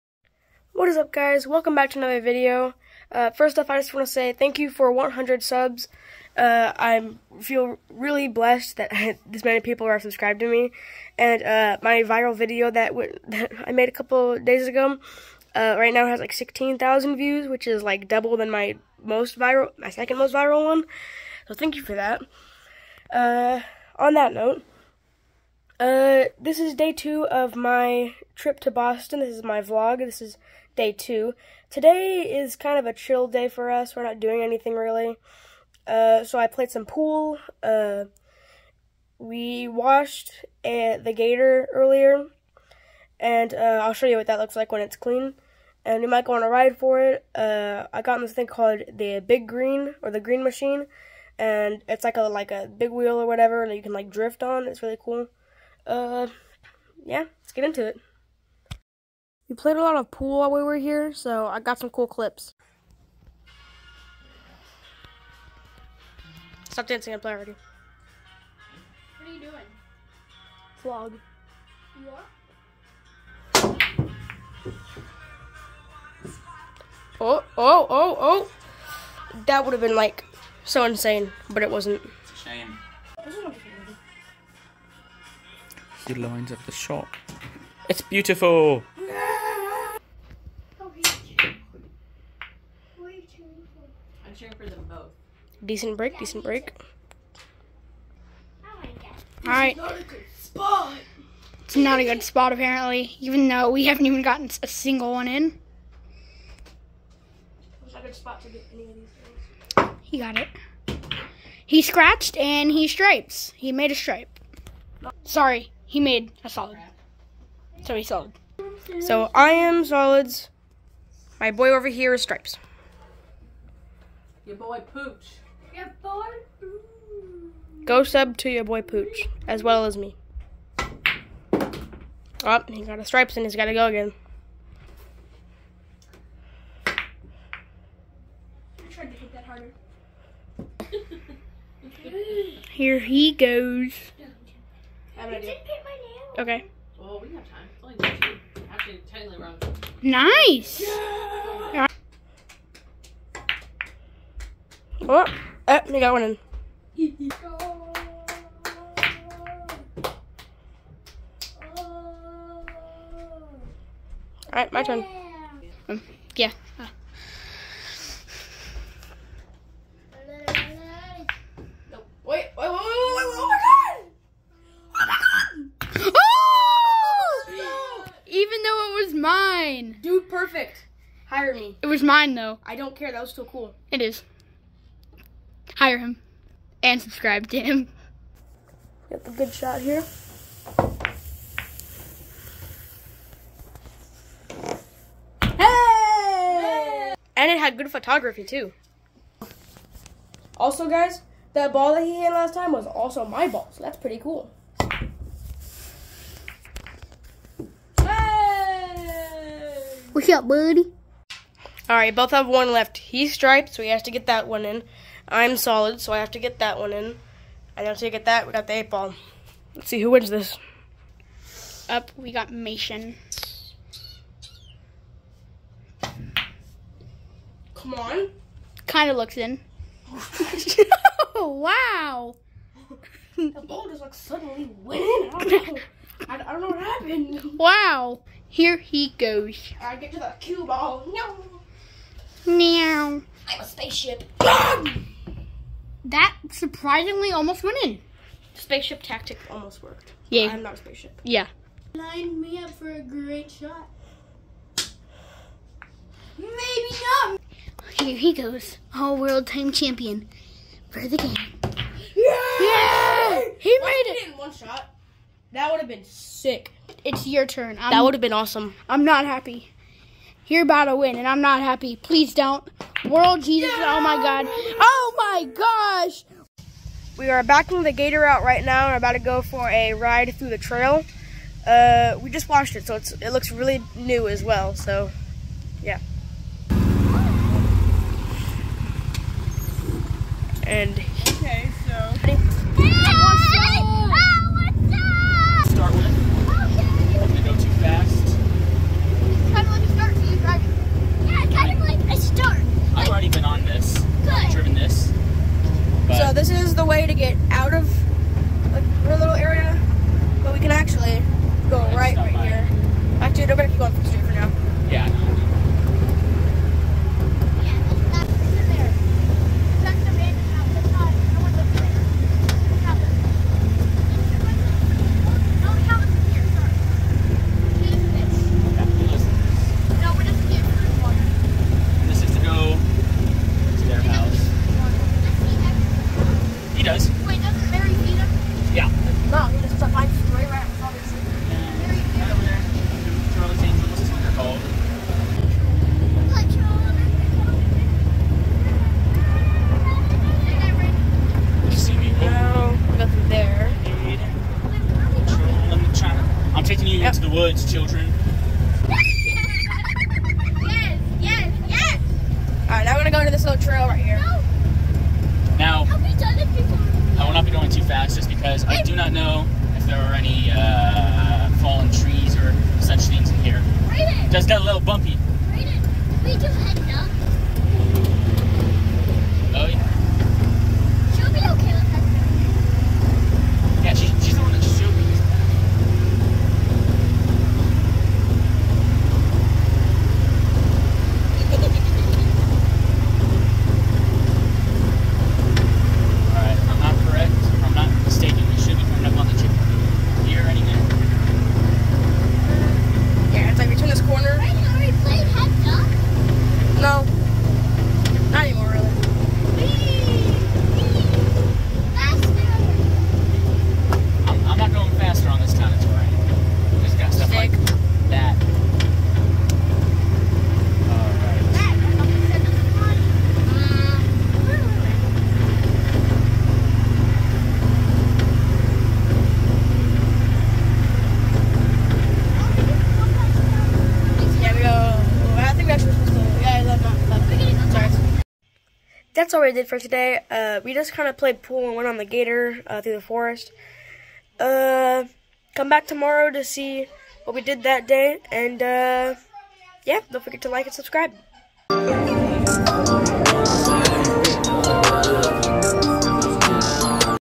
Boston Travel Vlog Day 2: Chilling and Games
This is day 2 of my Boston Travel Vlog. Today we hung out and played games. Come back every day to see what we did.